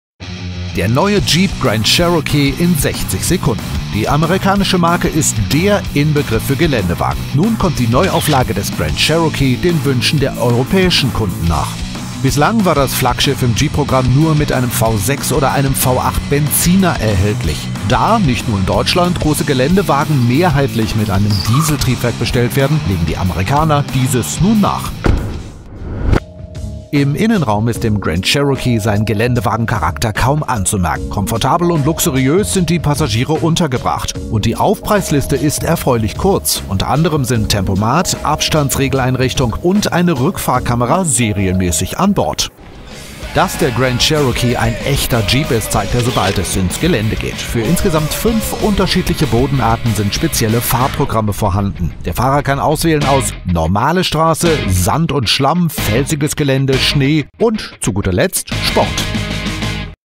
Jeep Grand Cherokee jetzt mit Diesel
Der Jeep wird 70 -- und die vierte Generation des Grand Cherokee wird runder. Erstmals unter der Haube des 2,3 Tonnen Koloss: ein Sechszylinder-Diesel mit drei Liter Hubraum mit 140 kW/190 PS und einem maximalen Drehmoment von 440 Newtonmetern ab 1.600 U/min, oder mit 177 kW/241 PS und satten 550 Newtonmetern Drehmoment. Viel Leistung mit weniger Verbrauch.Quelle:UnitedPictures.TV